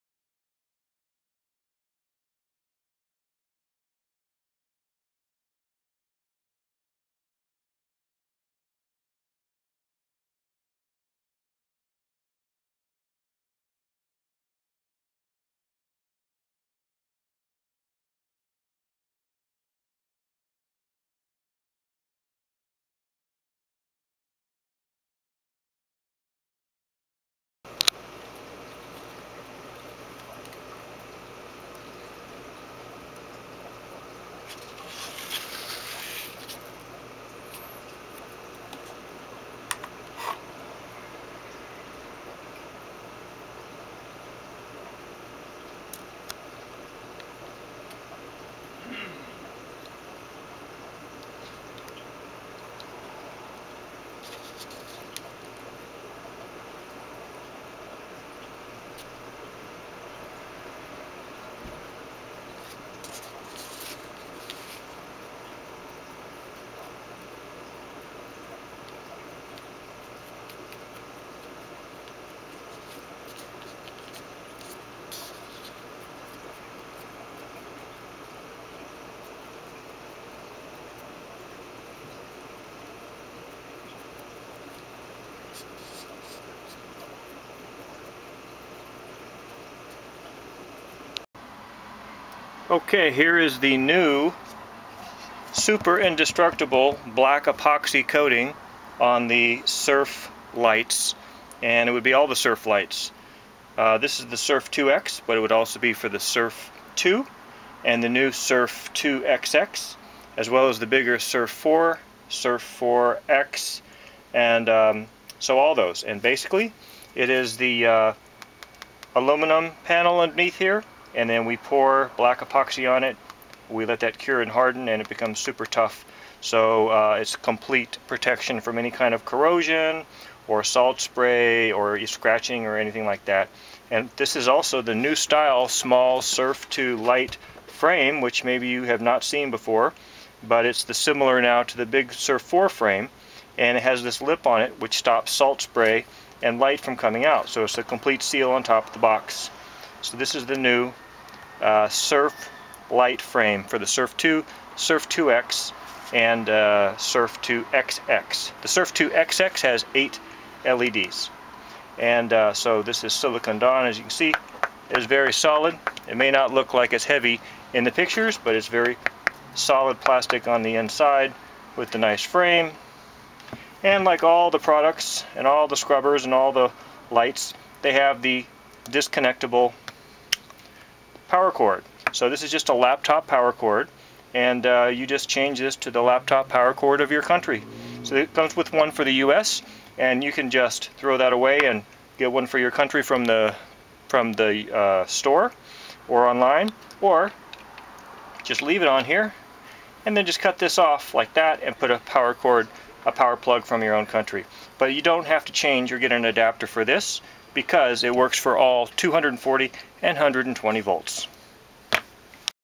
SURF scrubber improvements, including black epoxy coating
This video shows the new frame design on the SURF2, SURF2x and SURF2xx which blocks light and water from coming up over the light. Also it shows the black epoxy coating that now comes on all SURF models including the SURF4, SURF4x, SURF8 and SURF8x.

Videos of the smaller SURF4, which is the same as one compartment of the SURF8: